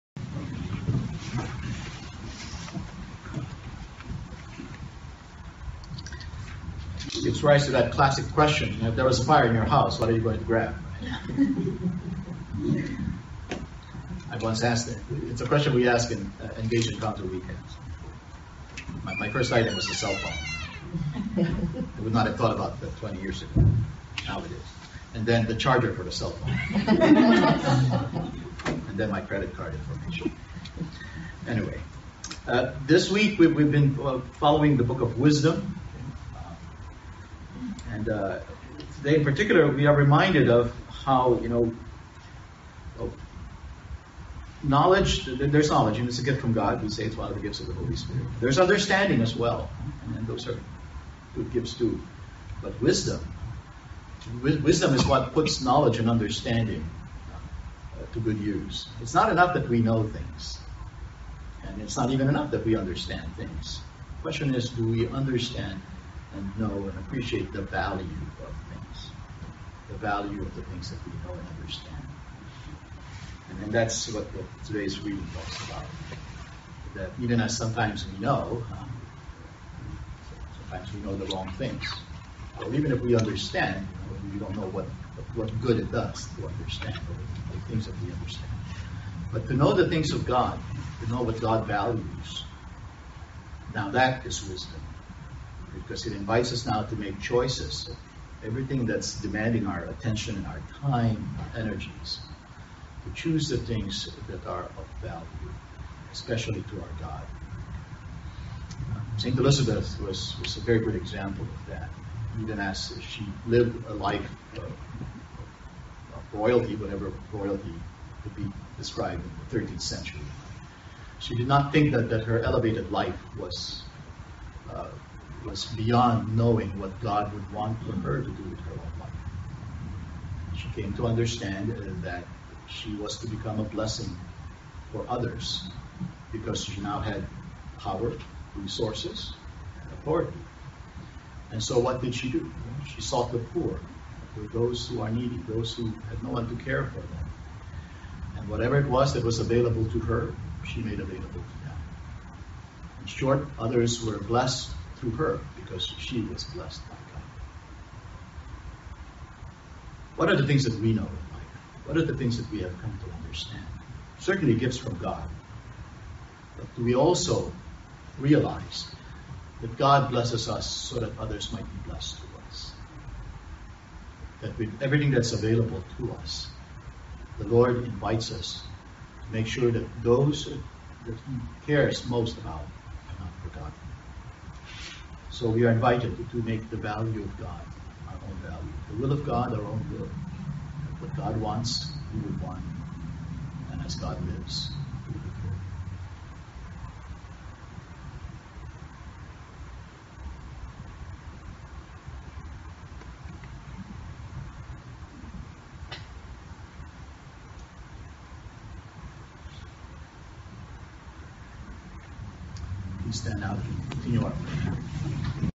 Henson ACC homily podcast 11-17-17: Wisdom=
Homily of Fr. Joel Henson at Archdiocese of Los Angeles Catholic Center (ACC) noon Mass 17 Nov. 2017: =know, choose what God values; bless the poor thanks to videographer Corita Reyes)